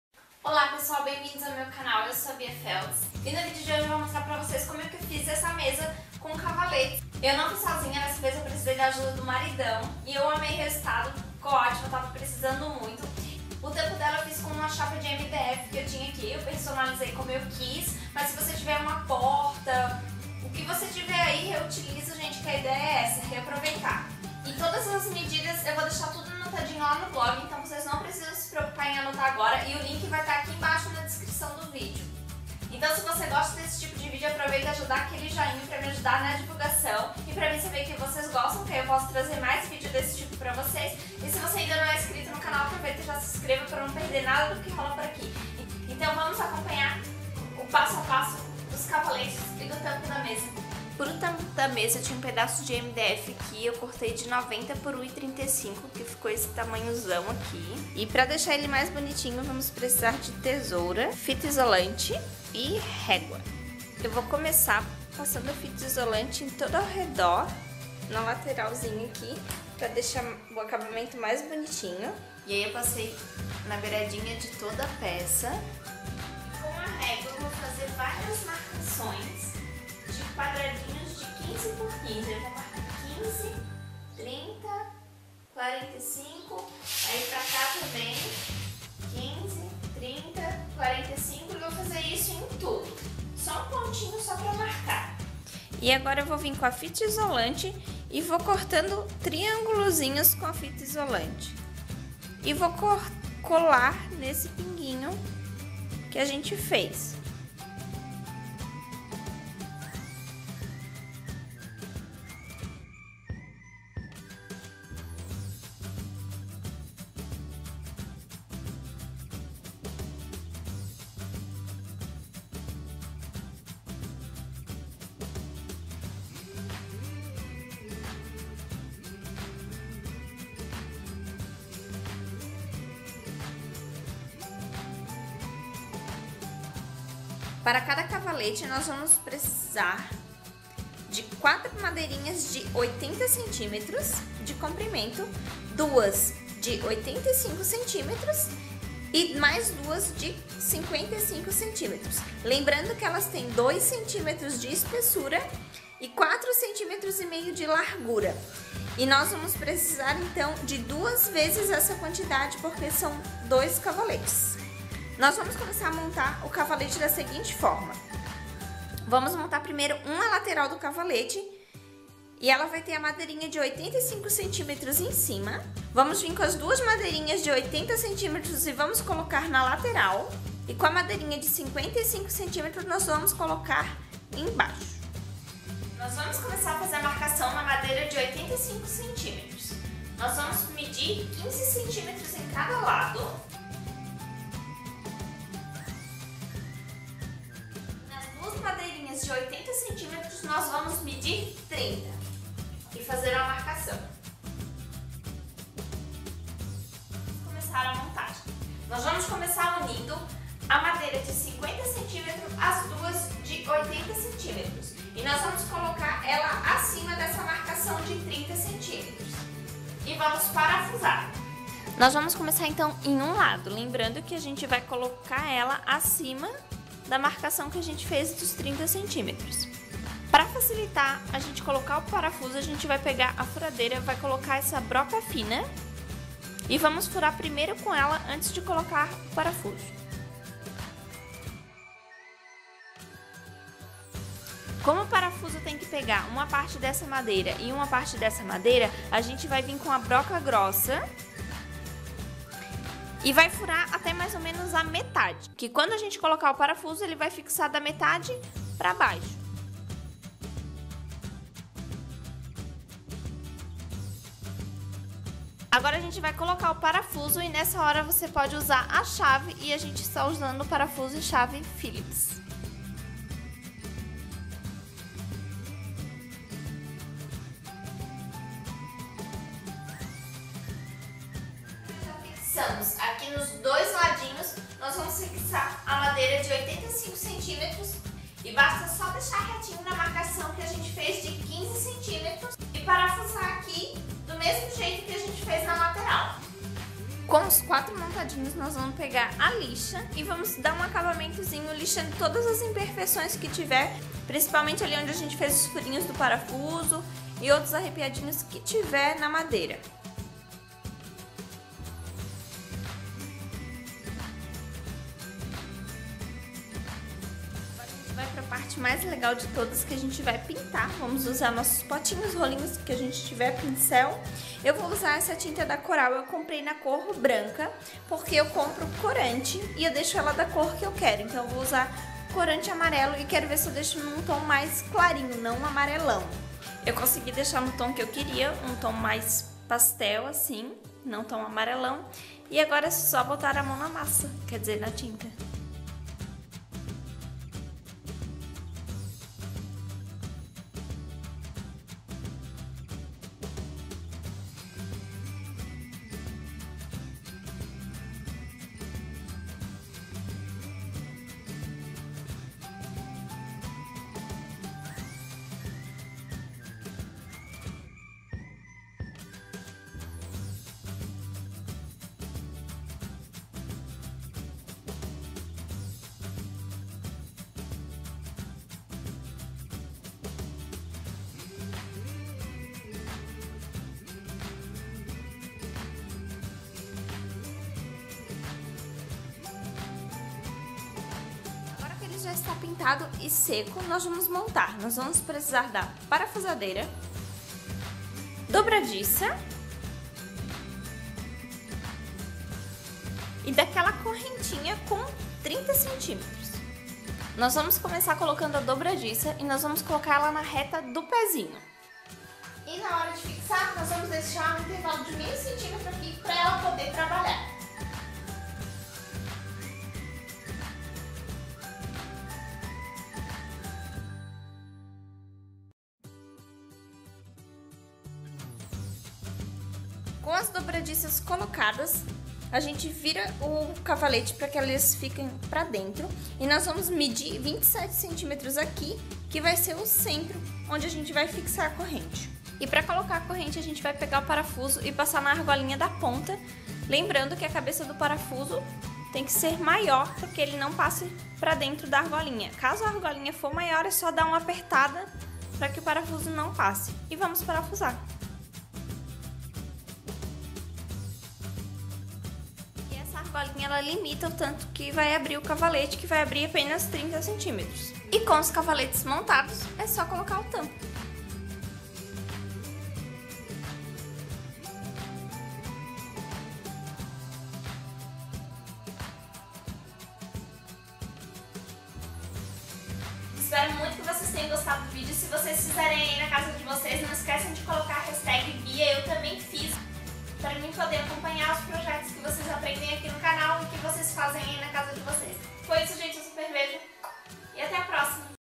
DIY | Mesa cavalete - Bia Feltz
Olá pessoal !!! Que tal fazer uma mesa que você pode desmontar quando precisar de espaço, ou trocar ela de ambiente com facilidade...